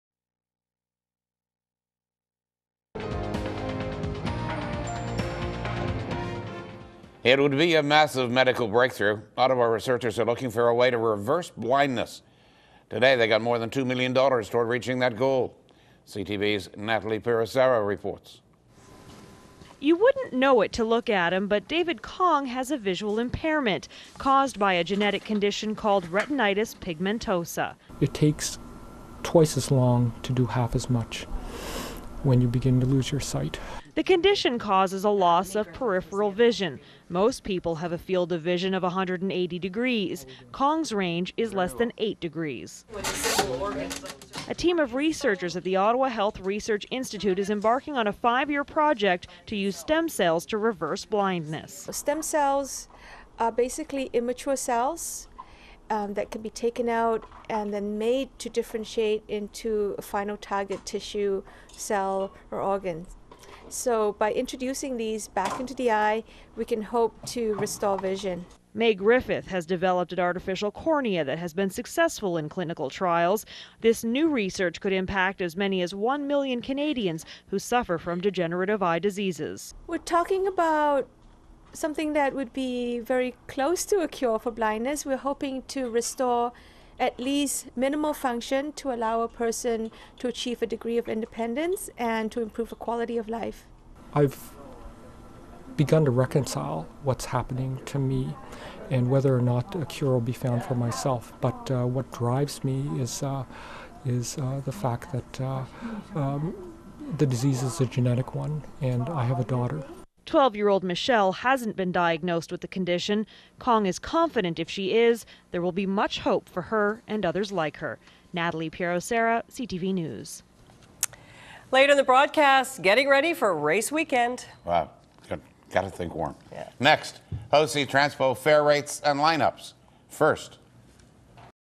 CTV - Reversing Blindness - Mar 4, 2009
Natalie Pierosara reports: Canada's leading researchers working together thanks to unique $2.4 million grant to develop stem cell therapies to reverse blindness.